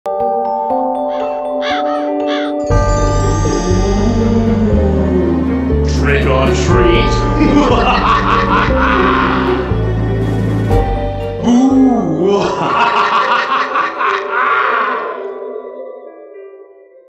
Haunted Hill Farm 2024 Pumpkin Man
COMING THIS SUMMER! This pumpkin man will give guests the fright of their lives with a single glance. With a touch or bump, he starts shaking while his face and chest glow in green LED lights. His 2 creepy phrases accompanied by sounds of cawing crows and mystical chimes will be sure to scare off passersby even from afar. Battery operated, he includes a stand and an optional hanging loop at the top of his head so you can display him however and wherever you want.